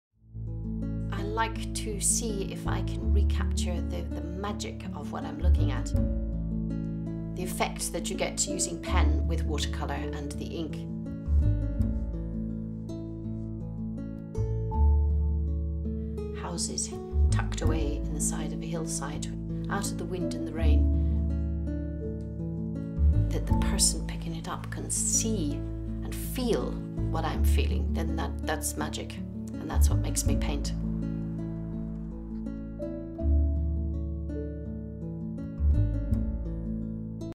Janet Vinnell Pastel Artist
A short video giving a little insight into the pastel artist Janet Vinnell. She is a Galway based artist and runs a small gallery in the centre of the city. The Black Cat Gallery has been open to the public for many years providing customers with a range of cards, ceramics and paintings for any special occasion.